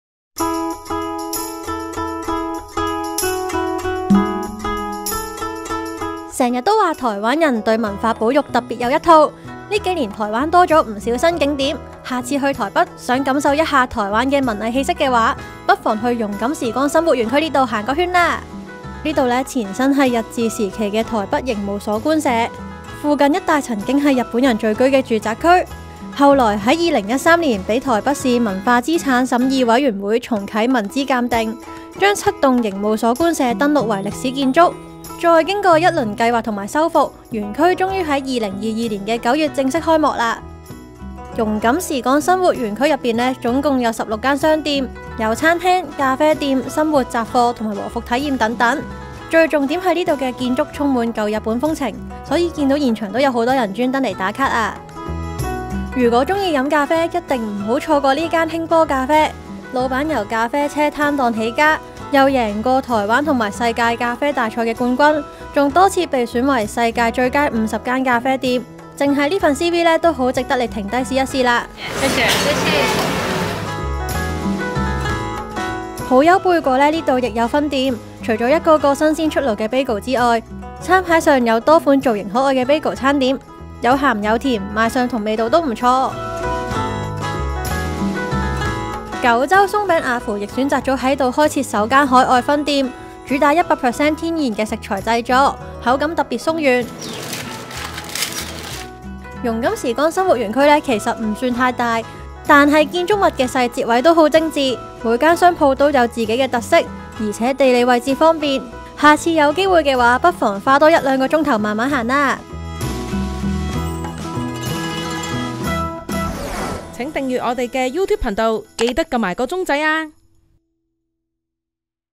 台灣自由行｜台北最新好去處！「榕錦時光生活」免費參觀日治時期建築 16間特色商店 世界冠軍級咖啡＋好丘貝果＋九州鬆餅瓦芙
台北新景點「榕錦時光生活」在2022年9月正式開幕，以日治時期臺北刑務所官舍改建而成，有近16間商店進駐，包括世界冠軍咖啡店、好丘貝果、和服租借等等，加上精緻富歷史感的日本建築，隨即成為台北最熱門的旅遊景點！

鳴謝：
臺灣觀光協會香港辦事處
SONY HK提供攝影器材

台北免費景點！日治時期刑務所官舍
幾年都沒有機會到台北旅遊，香港人或者對覺得「榕錦時光生活」這個名字很陌生，如果要舉個相近的例子的話，「榕錦時光生活」這裡其實類近香港的大館，同樣是以監獄設施改建，在保留建築特色的同時，邀請了多間特色商店進駐。「榕錦時光生活」前身是臺北刑務所官舍，是臺灣最大規模監獄「原臺北刑務所」的獄政官員宿舍，附近一帶曾是日本人聚居的住宅區，後來在2013年被臺北市文化資產審議委員會重啟文資鑑定，將七棟刑務所官舍登陸為歷史建築，再經過一輪計劃和修復，園區終於在去年正式開幕。

世界冠軍級咖啡 好丘貝果選物店
園區內有多間商店，如果喜歡飲咖啡，一定不能錯過興波咖啡。老闆是個咖啡痴，在2009年以咖啡車攤檔起家，從2013年起連續三年拿下台灣咖啡大師賽冠軍，並於2016年拿下WBC世界咖啡大賽冠軍，店舖更多次被選為「世界最佳50間咖啡」，單是這份履歷都值得你停下來一試。招牌貨是充滿台灣元素的「南島煙燻冰咖啡」，在咖啡中加入愛玉、冬瓜茶、桂花釀，搭配本身帶有花香、黑糖香氣的水洗沃卡咖啡豆，組合非常特別。台灣人氣品牌好丘貝果在這裡亦設有分店，除了一個個新鮮出爐的貝果麵包，餐牌上有多款造型可愛的貝果餐點，有鹹有甜賣相味道都不錯，可加配台式特飲，簡單美味。另外九州鬆餅瓦芙首間海外分店亦不能不試，品牌在日本相當有名氣，嚴選九州特色七種穀物製成，口感比一般鬆餅鬆軟Q彈。

榕錦時光生活園區
地址：台北金華街135號至177號及金山南路2段112號

bmgproductionmusic